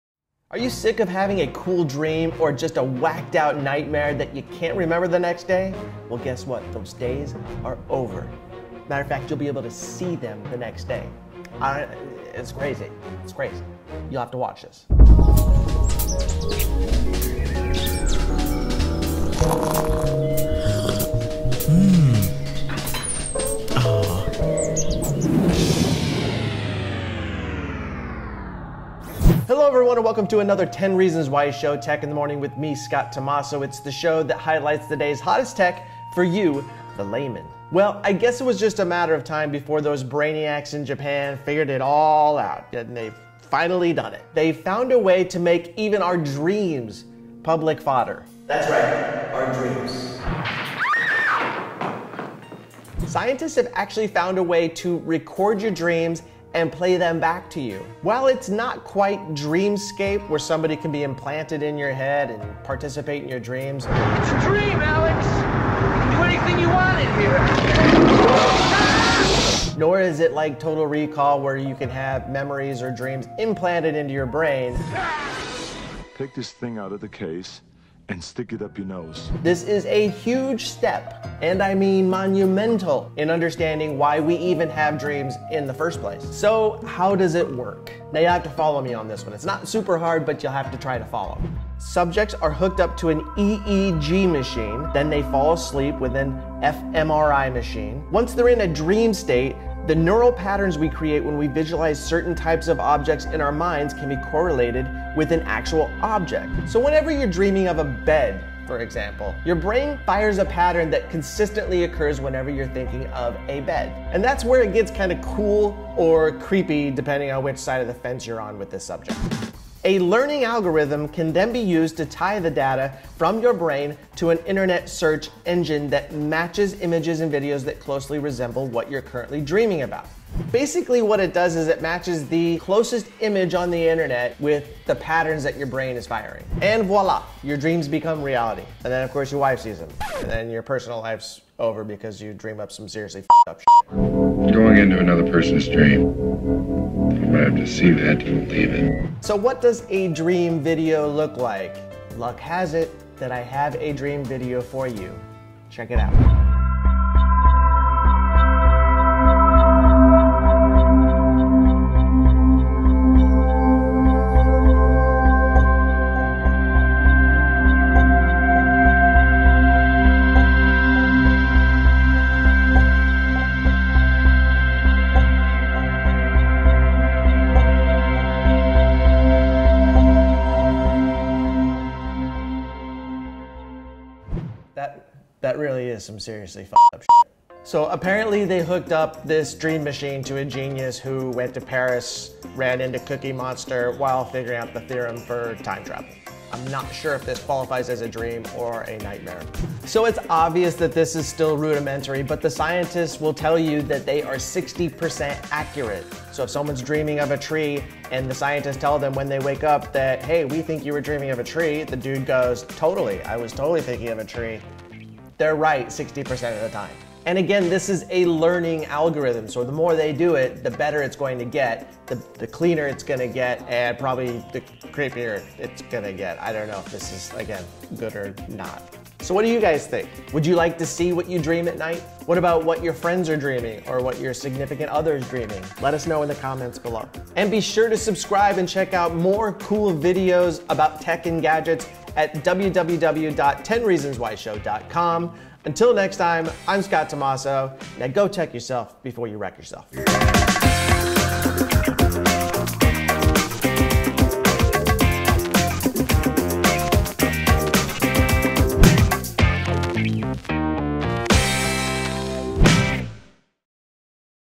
VIDEO of REAL DREAM Recorded by New "Dream Machine"
AUDIO FIXED!

Are you sick of having a cool dream or nightmare that you can't remember the following morning? Well those days... are over...

Hello everyone and welcome to another 10 Reasons Why Show: Tech in the Morning with me Scott Tomasso. It's the show that highlights the day's HOTTEST tech for you... the layman.

Well... I guess it was just a matter of time, but those brainiacs over in Japan have finally done it. They've found a way to make even our DREAMS available for public fodder. That's right, our DREAMS!

Scientists have literally found a way to record your dreams AND play them back to you. While it's not quite "Dreamscape" or "Inception" where someone else can be planted inside your head and actively participate in your dreams, nor is at the level of "Total Recall" where dreams or memories can be implanted into your head, it IS a huge -- and I mean HUGE step - in the understanding of why we have dreams in the FIRST PLACE... and eventually make money off it in the future (duh!).

So how does this work? Subjects were hooked up to an EEG machine, then fell asleep with an fMRI machine. Once in a dream state, the neural patterns the subject creates when he or she visualizes certain types of objects in his mind can be correlated with an actual object. So if you're dreaming of a bed, your brain fires in a pattern that consistently occurs whenever you think of a bed. That's when it gets cool, or creepy -- depending on which side of the fence you're on with this subject. A "learning" algorithm can then be used to tie the data from your brain to an internet search engine that "matches" images and videos that closely resemble what you're currently dreaming about.  And voila! Your dreams become reality and in turn your wife sees them and your personal life is ruined because you dream some seriously f@cked up sh%t.

What does a dream movie look like? Well, here you go...

Apparently the scientists hooked up their dream machine to some genius who went to Paris and saw Cookie Monster while figuring out the theorem for time travel.

Obviously the "dream-machine" is still rudimentary, but the scientists who use it claim they have a 60% success rate when they tell the subject what he or she was dreaming about. And again, this is a learning algorithm so it will only get better, the pictures will get even more clearer, and it will probably get a little more creepier. I don't know.

So what do you guys think? Do you want to see what you dream at night? What your friends or significant other dreams?

Until next time, I'm Scott Tomasso. Now go tech yourself before you wreck yourself.